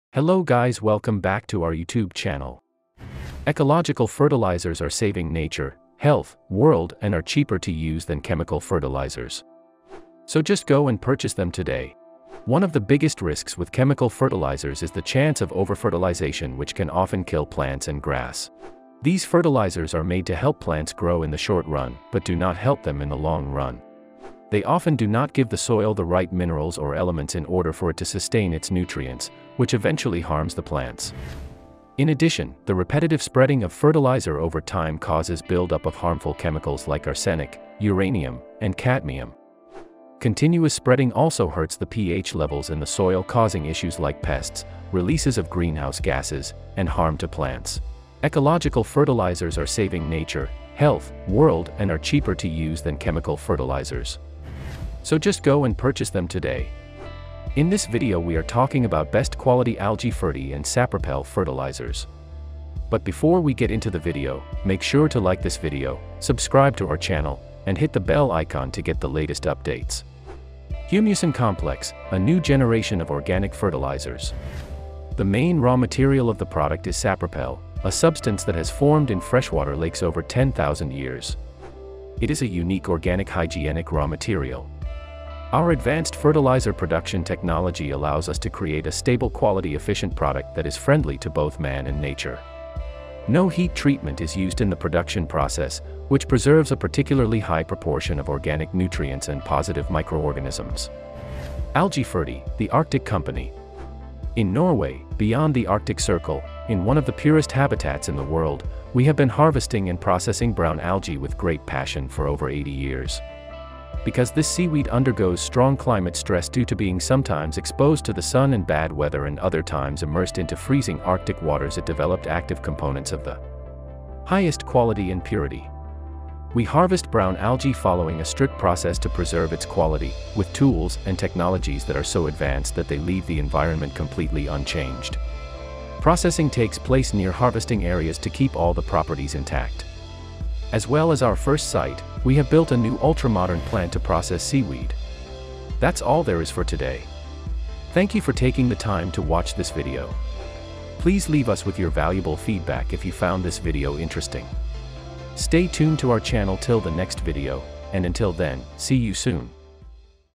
Best Quality Algae and Sapropel Eco Fertilizers
Ecological Fertilizers are saving nature, health, World and are cheaper to use
One of the biggest risks with chemical fertilizers is the chance of overfertilization which can often kill plants and grass. These fertilizers are made to
help plants grow in the short run, but do not help them in the long run. They
often do not give the soil the right minerals or elements in order for it to sustain
its nutrients, which eventually harms the plants. In addition, the repetitive
spreading of fertilizer over time causes build up of harmful chemicals like
arsenic, uranium, and cadmium. Continuous spreading also hurts the pH levels
in the soil causing issues like pests, releases of greenhouse gases, and harm to
plants.
In this video we are talking about Best Quality Algae and Sapropel Fertilizers.
HUMUSON COMPLEX, a new generation of organic fertilizers. The main raw
material of the product is Sapropel, a substance that has formed in freshwater
lakes over 10,000 years. It is a unique organic hygienic raw material. Our
advanced fertilizer production technology allows us to create a stable quality
efficient product that is friendly to both man and nature. No heat treatment is
used in the production process, which preserves a particularly high proportion
of organic nutrients and positive microorganisms.
Algae, the Arctic Company.
In Norway, beyond the Arctic Circle, in one of the purest habitats in the world,
we have been harvesting and processing Ascophyllum nodosum with great
passion for over 80 years.
Because this seaweed undergoes strong climate stress due to being
sometimes exposed to the sun and bad weather and other times immersed into
freezing Arctic waters it developed active components of the
highest quality and purity. We harvest Ascophyllum nodosum following a strict
process to preserve its quality, with tools and technologies that are so
advanced that they leave the environment completely unchanged.
Processing takes place near harvesting areas to keep all the properties intact.
As well as our first site, we have built a new ultra-modern plant to process
seaweed.
That's all there is for today. Thank you for taking the time to watch this video.
Please leave us with your valuable feedback if you found this video interesting.